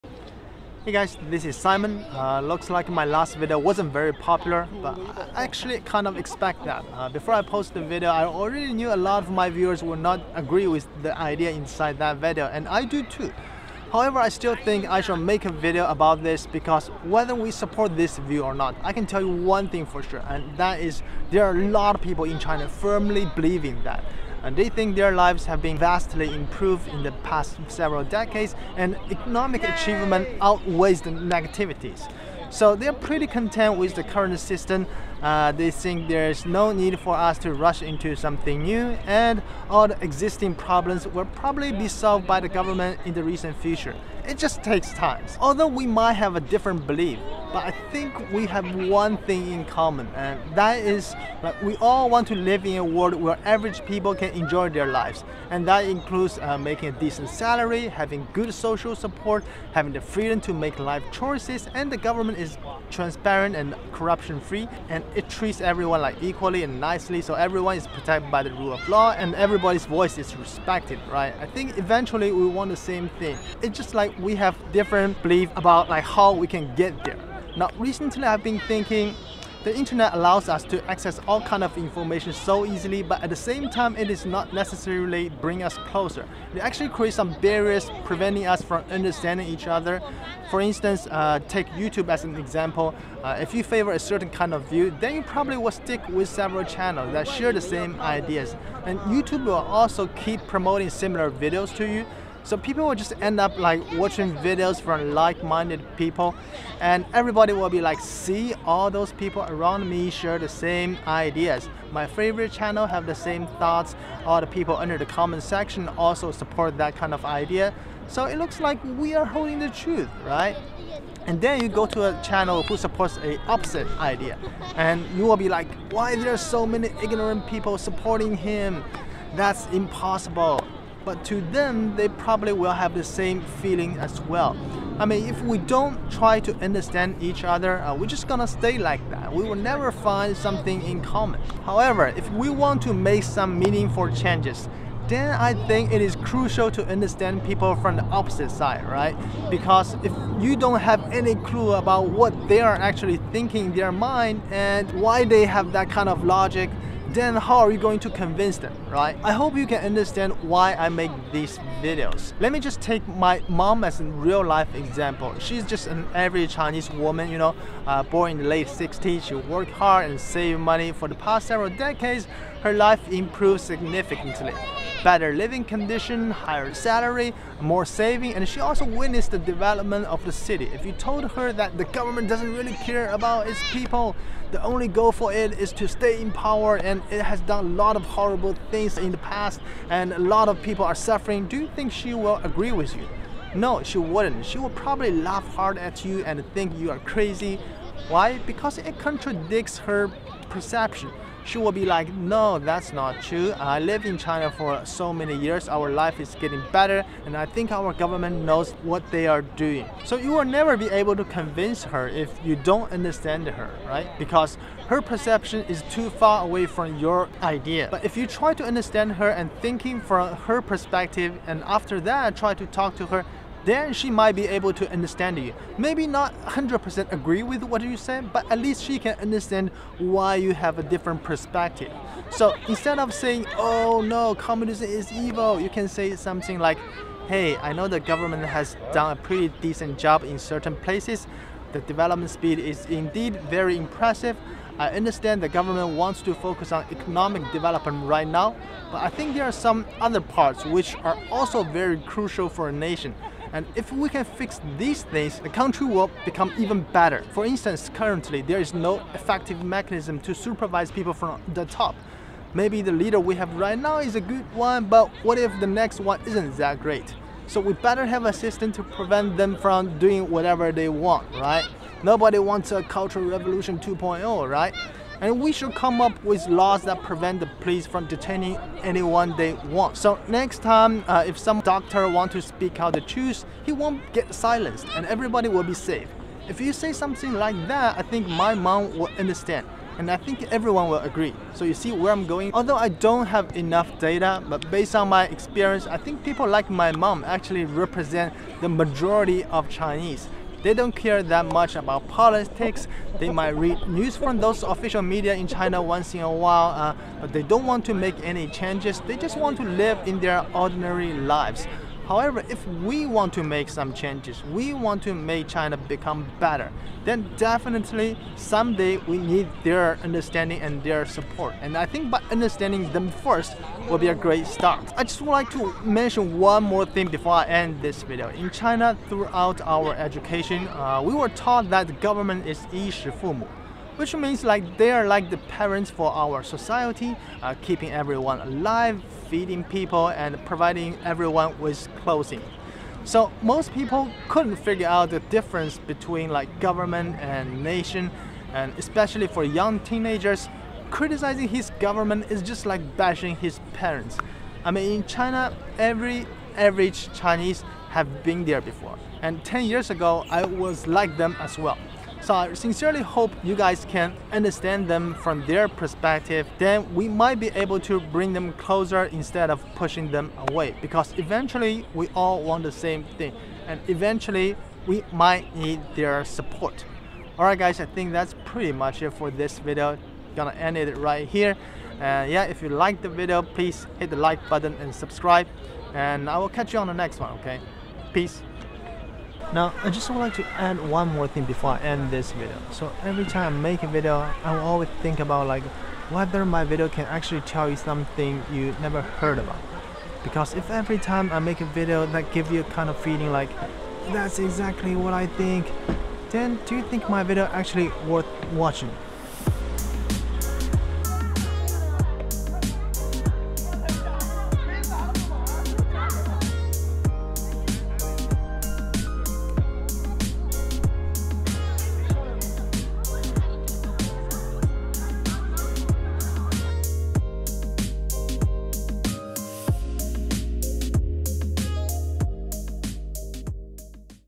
People who support China's system | why we should try to understand them | how to communicate
Turn on subtitles in order to understand everything we said in the video.
Once again, thank you so much for everyone's love and support.:)